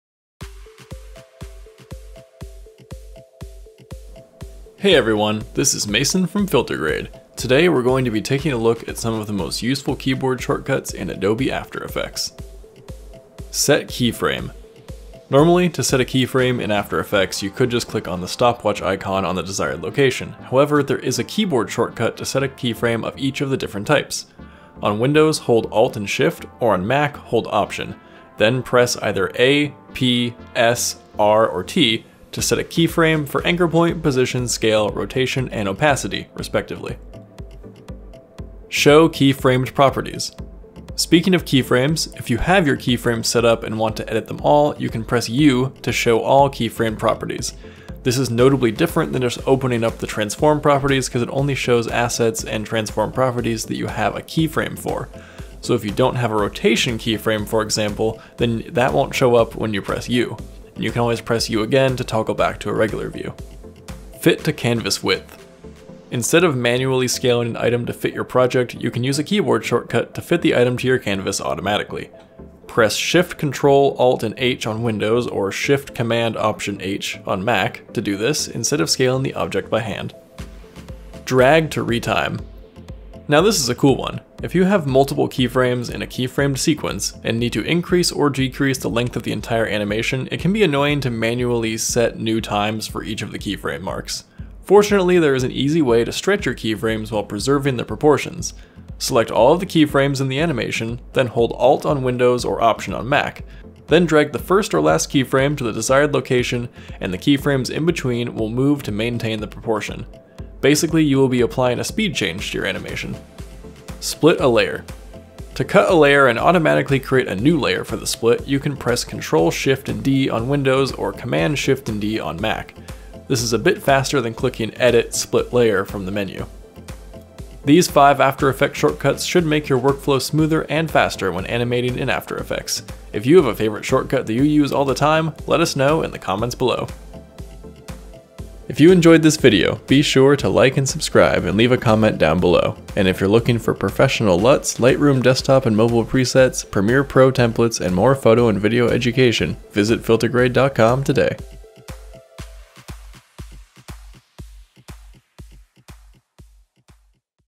5 TIME SAVING AFTER EFFECTS SHORTCUTS
Taking a look at some of the most useful keyboard shortcuts in Adobe After Effects. Save time when editing videos and adding keyframes with these helpful shortcuts!

After Effects Shortcuts covered in this video:

1. Set Keyframe
2. Show Keyframed Properties
3. Fit to Canvas Width
4. Drag to Retime
5. Split a Layer

These 5 After Effects shortcuts should make your workflow smoother and faster when animating in After Effects.

Have any questions? Want us to cover a new topic for video editing in the next video?

Follow FilterGrade!